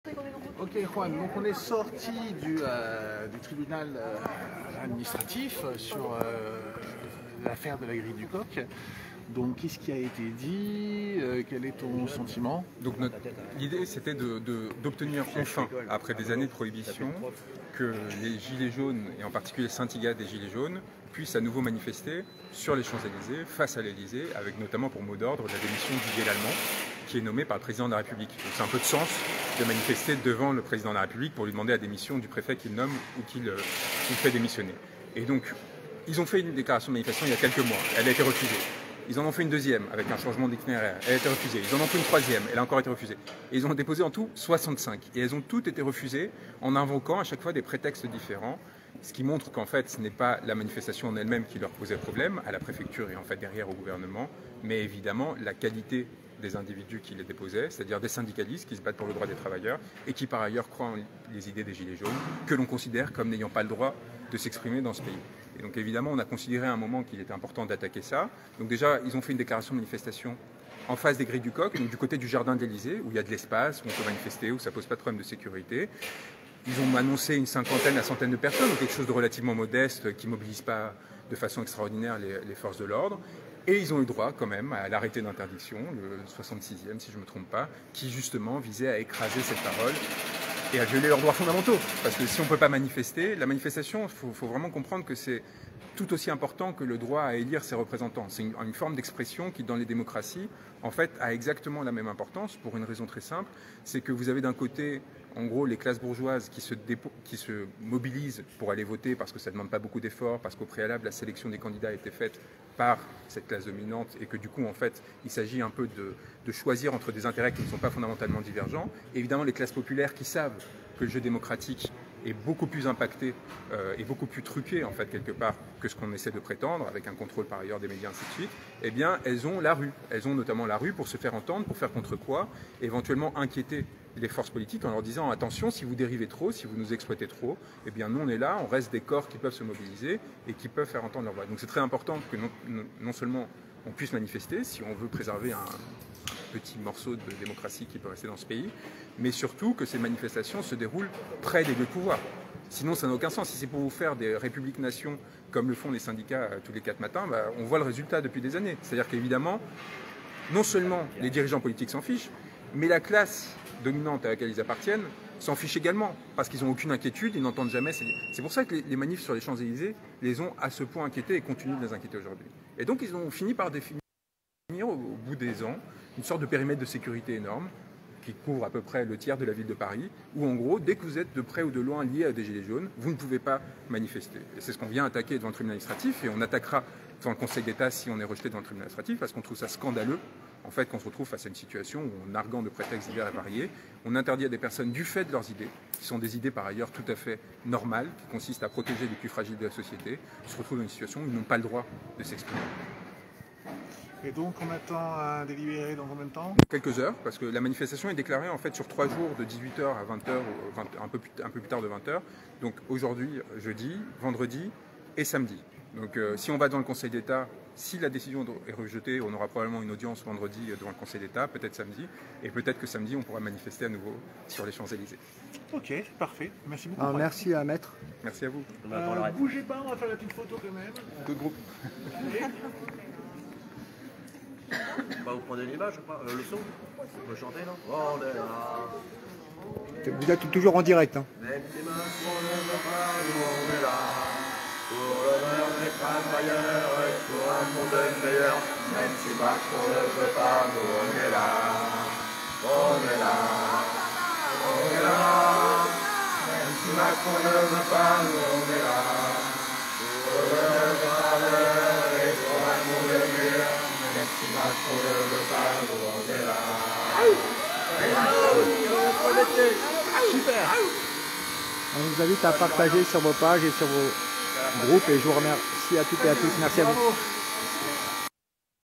Juan Branco - Les Gilets Jaunes et les Champs Élysées
Nous nous demandions pourquoi les amendes pleuvaient sur les Champs.